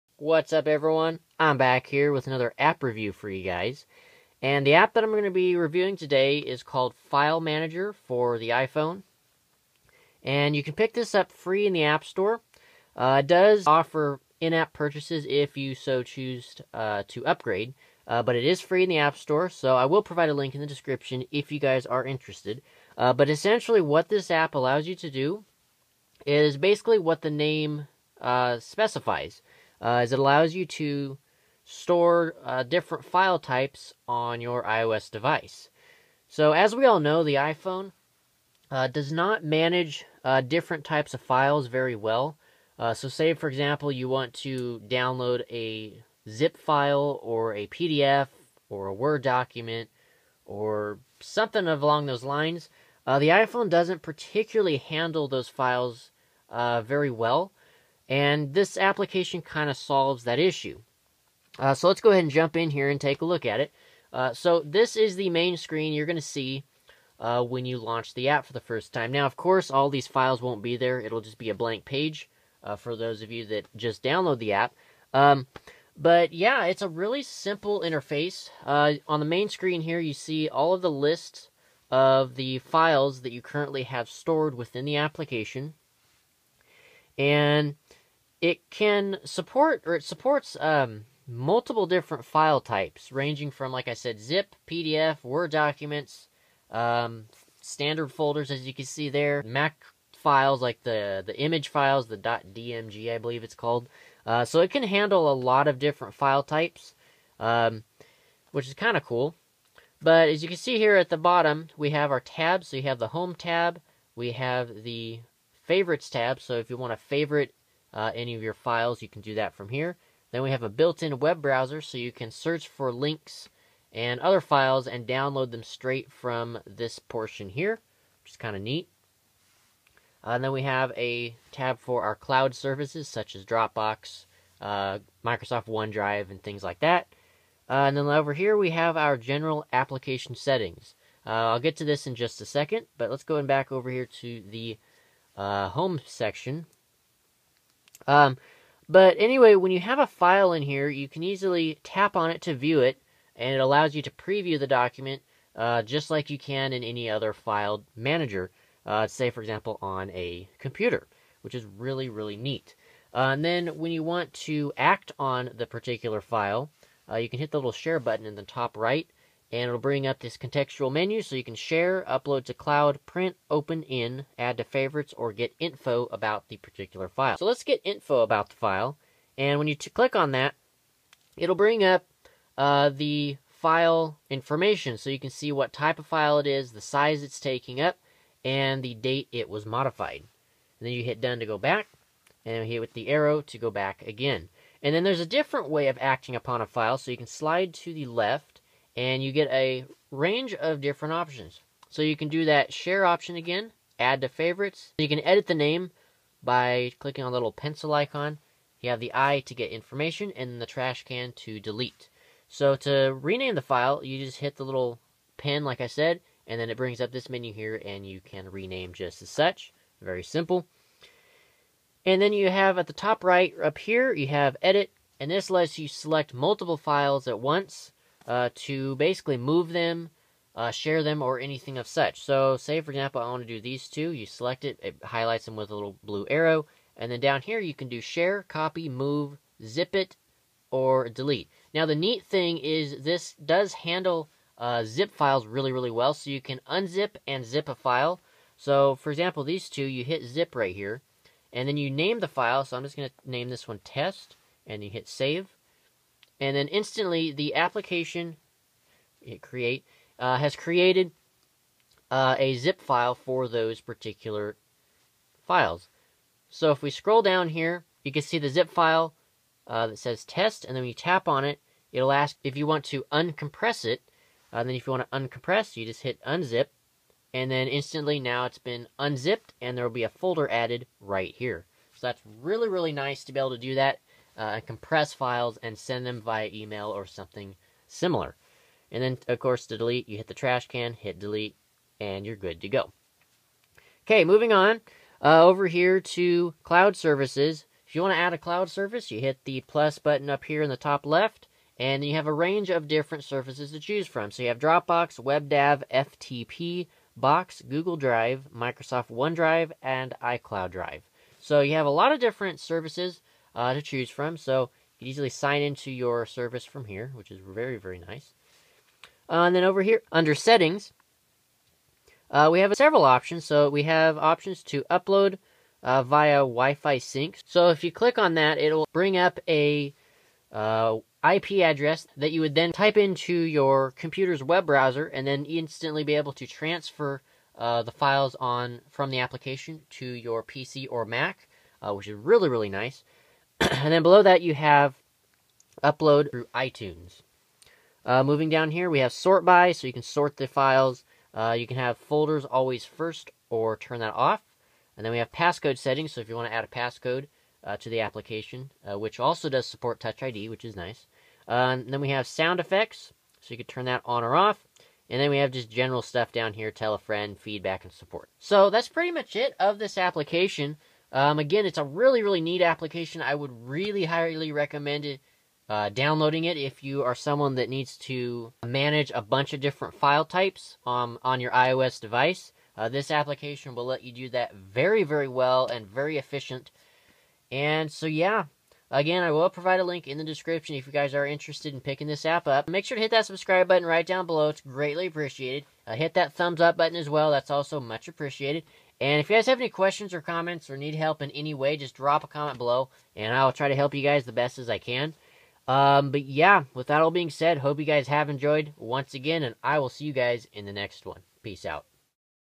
App Review - File Manager - File Management App for iOS
In this video I take a look at File Manager for iOS. This app is free in the Apple App Store however, there is an in-app purchase that is required to gain access to some features. I'd highly recommend checking this app out!